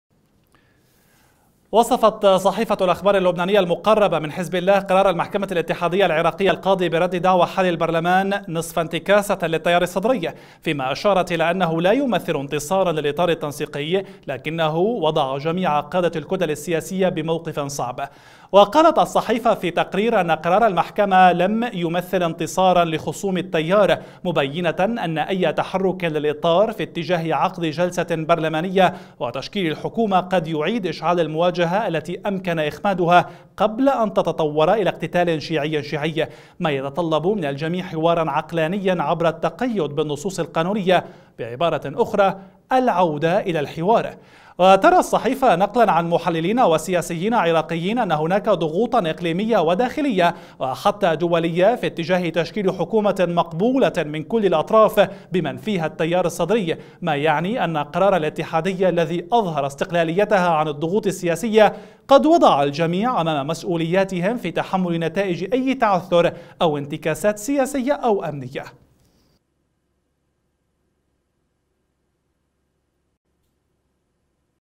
الاخبار اللبنانية تصف قرار الاتحادية بنصف انتصار للتيار والاطار
وصفت صحيفة الأخبار اللبنانية المقربة من حزب الله قرار المحكمة الاتحادية العراقية القاضي بردّ دعوى حلّ البرلمان،نصف انتكاسة للتيّار الصدري، فيما أشارت إلى أنه لا يمثل انتصاراً للإطار التنسيقي، لكنه وضع جميع قادة الكتل السياسية بموقف صعب، وقالت الصحيفة في تقرير إن قرار المحكمة لم يمثل انتصاراً لخصوم التيار مبينة أن أيّ تَحرّك للإطار في اتجاه عقد جلسة برلمانية وتشكيل الحكومة قد يعيد إشعال المواجهة التي أمكن إخمادها قبل أن تتطَور إلى اقتتال شيعي – شيعي، ما يتطلب من الجميع حوارا عقلانيا عبر التقيّد بالنصوص القانونية؛ بعبارة أخرى العودة الى الحوار،
وترى الصحيفة نقلا عن محللين وسياسيين عراقيين أن هناك ضغوطاً إقليمية وداخلية وحتى دولية في اتجاه تشكيل حكومة مقبولة من كل الأطراف بمن فيها التيار الصدري، ما يعني أن قرار الاتحادية الذي أظهر استقلاليتها عن الضغوط السياسية قد وضع الجميع أمام مسؤولياتهم في تحمل نتائج أي تعثر أو انتكاسات سياسية أو أمنية.